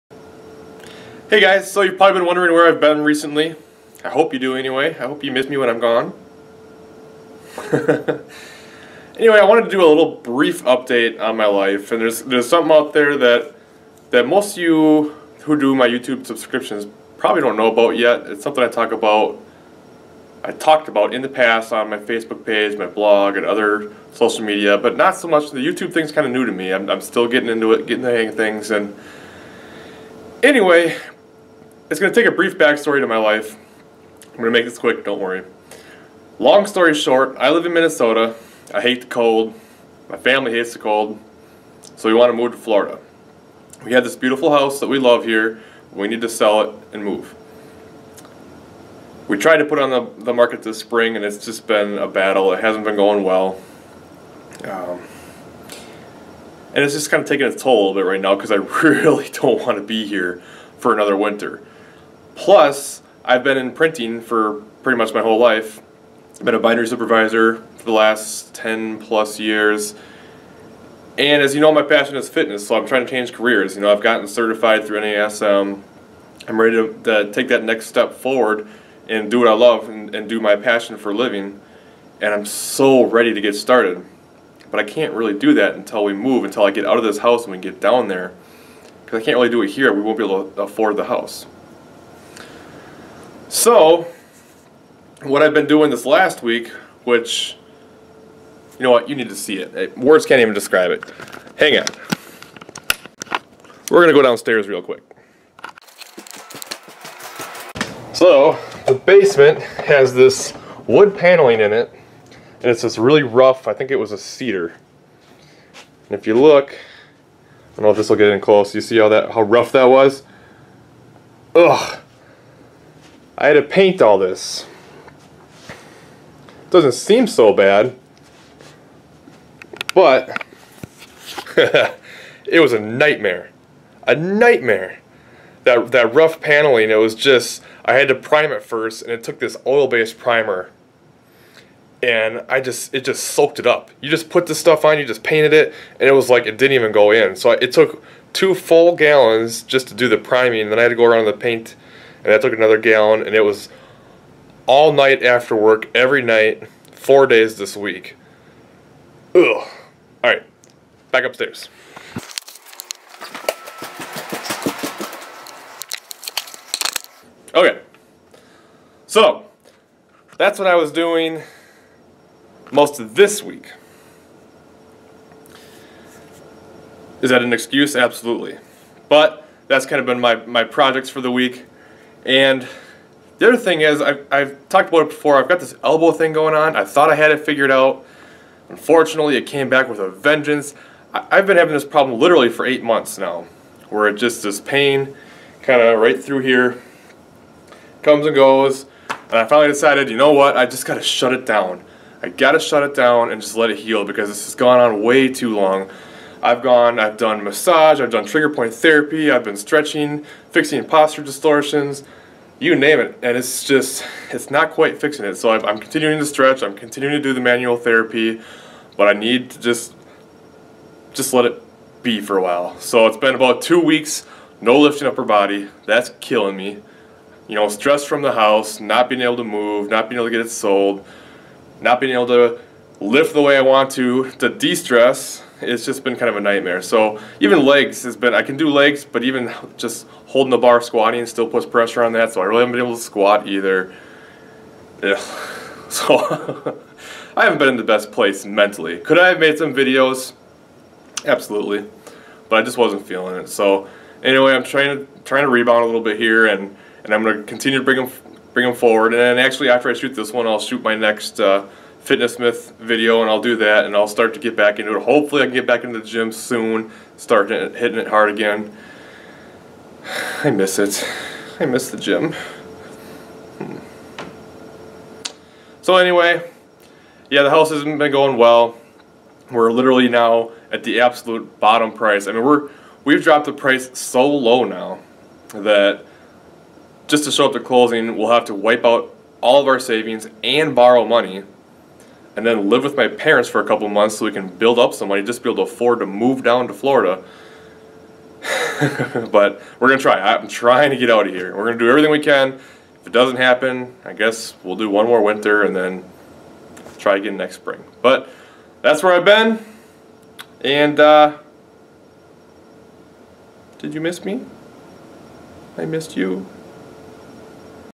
Where Has Colin Been?
I know I haven't been producing videos much or commenting on your videos. Here is why. Don't worry I'm coming back!